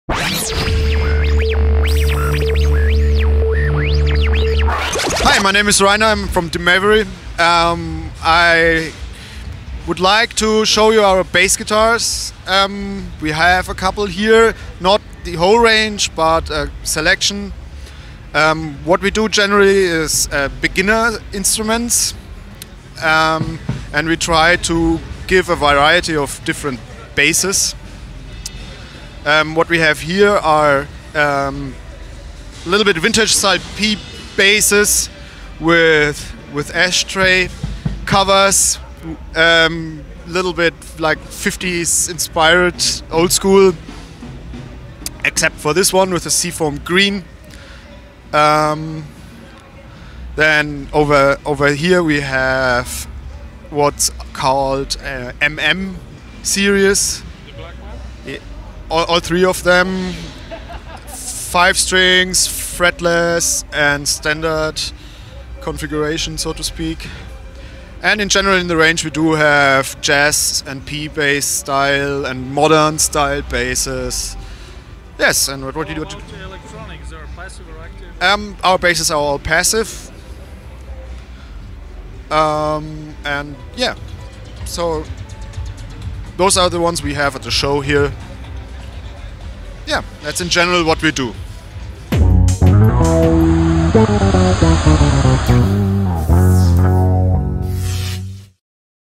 Musicpark Leipzig 2019: Dimavery
A report from the Dimavery booth at Musicpark Leipzig exhibition. - this is where everybody is obsessed with bass!

Between the 1st and the 3rd of November 2019, the first edition of the Musicpark exhibition was held in the Leipzig fairgrounds.

One of the exhibitors at the event was Dimavery, whose representative is about to tell you a thing or two about the bass guitars made by the German guitar and bass producer...

"Hi! My name is Reiner and I’m from Dimavery. I would like to show you our bass guitars. We have a couple here. Not the whole range but a selection. Generally, we make beginner instruments and we try to give a variety of different basses.

What we have here are vintage-style P-Basses with ashtray covers. A little bit 50s-inspired old school. Except for this one, in the Sea Foam Green finish.

Over here, we have the MM series - all three of them. There’s the 5-string, fretless and a 4-string. In general, we offer J-Bass, P-Bass and modern style bass guitar configurations.

As for electronics, our basses are all passive. Those are the ones we have at the show here and that’s in general what we do."